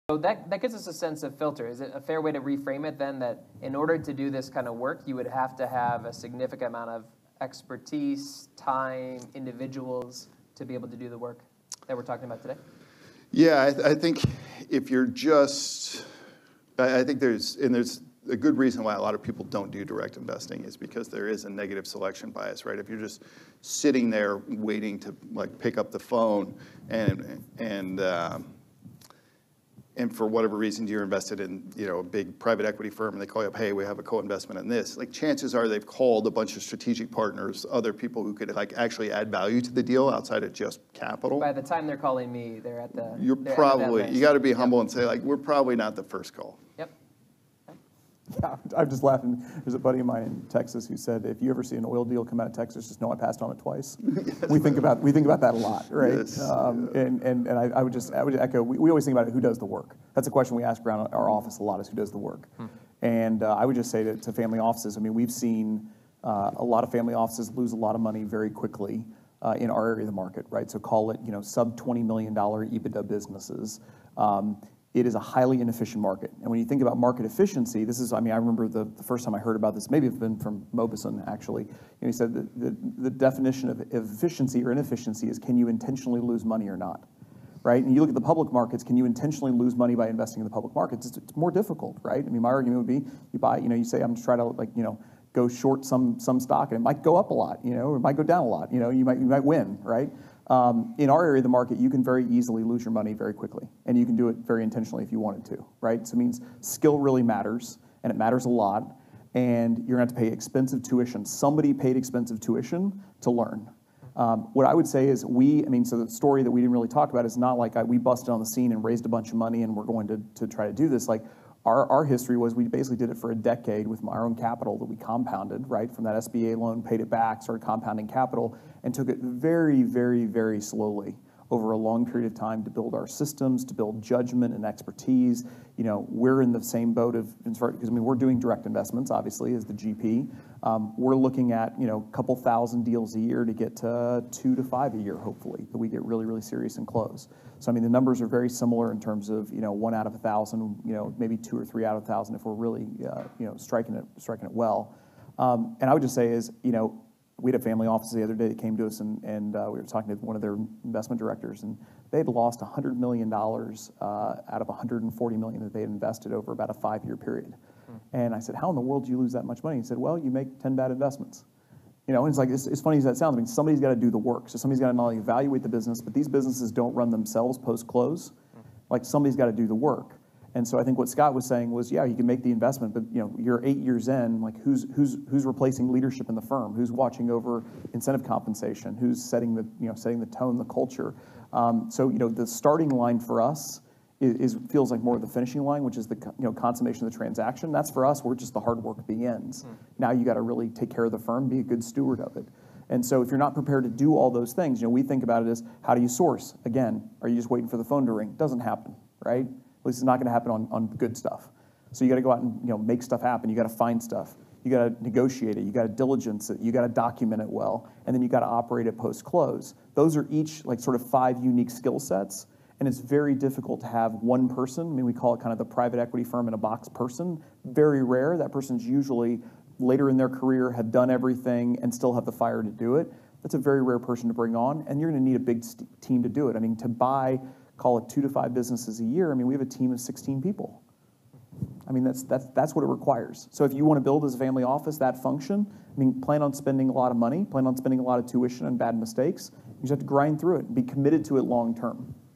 On Negative Selection Bias in Direct Investment - Scott Wilson and Brent Beshore
This excerpt was taken from the Family Investment and Impact Series: Strategies for Direct Investment. This virtual event was held on 12/01/2021 at Washington University in St. Louis.

Learn more about the Koch Center for Family Business and our upcoming events

In the first of three sessions in WashU Olin’s inaugural Family Investment and Impact Series, we examined the topic of direct investment strategy by families and family offices. The event featured Scott Wilson, CIO of Washington University Investment Management Company, fresh off of his team’s industry-leading performance in the university endowment space. He was joined by long-term investor and finance guru Brent Beshore, CEO, and founder of Permanent Equity. The two spoke about direct and fund investment strategies and the capabilities required by families and family offices to do this work effectively. The session was moderated by Koch Professor of Practice for Family Enterprise Peter Boumgarden.